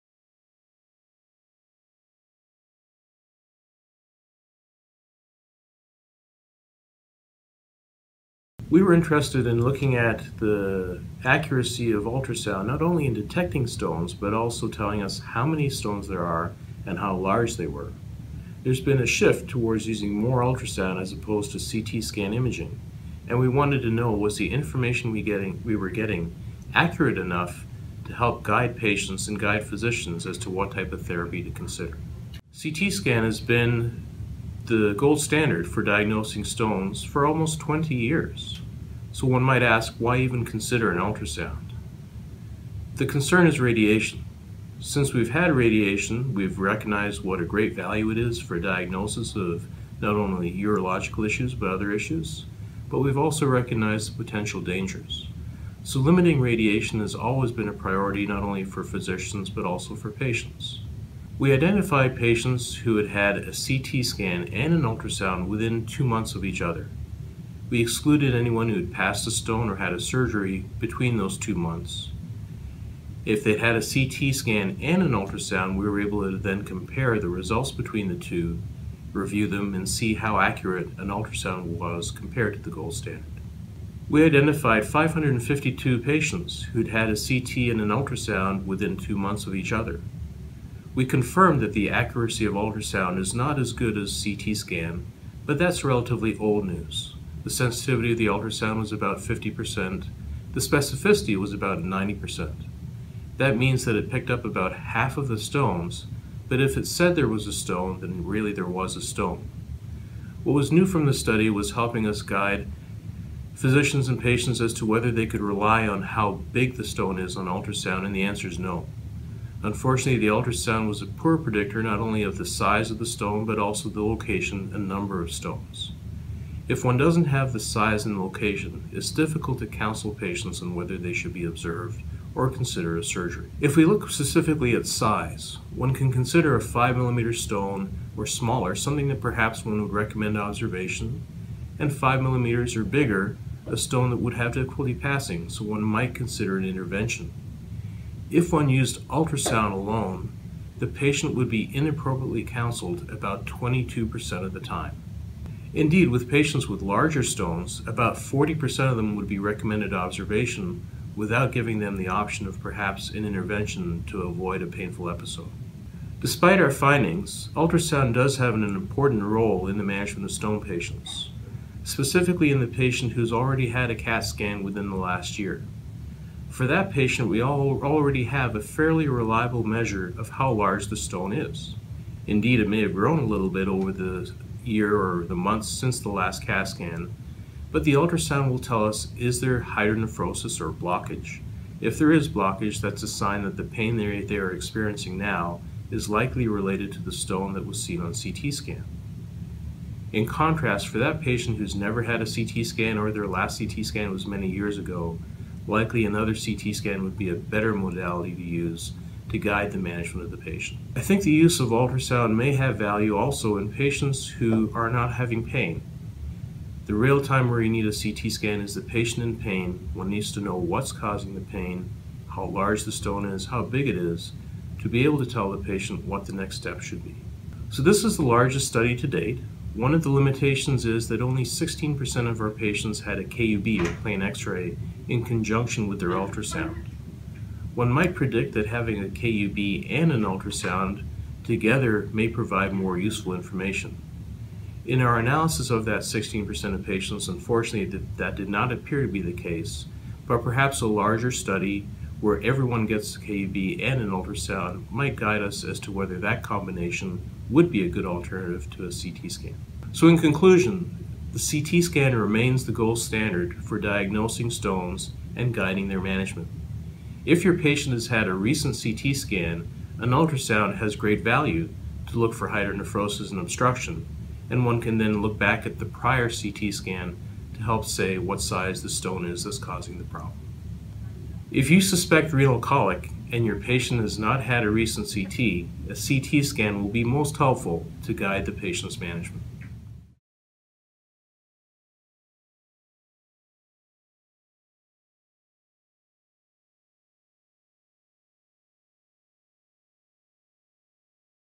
Accuracy of US for renal stone detection and size determination
Accuracy of ultrasonography for renal stone detection and size determination: is it good enough for management decisions?

Vishnu Ganesan1,2, Shubha De1, Daniel Greene1, Fabio Cesar Miranda Torricelli1 andManoj Monga1,*

DOI: 10.1111/bju.13605